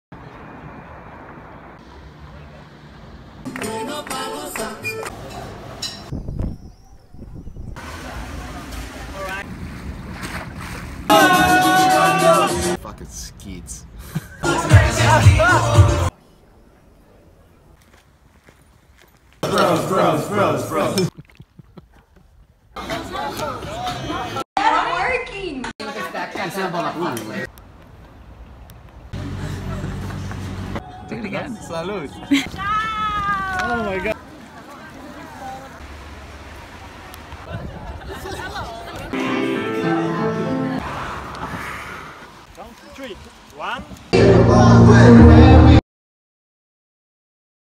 Remote Year Kaizen - Month 4 - Sofia, Bulgaria
One Second Everyday from Kaizen's Month 4 in Sofia, Bulgaria.  Guest appearances by Spain and Italy.